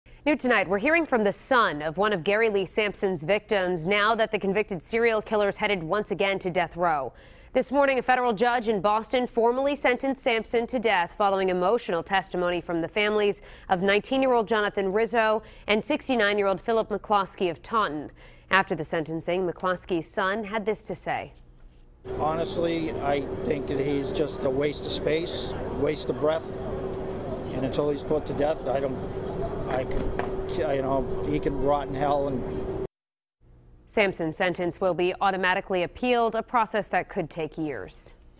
Gary Lee Sampson Sentenced to Death
The families of the victims offered emotional testimony in court.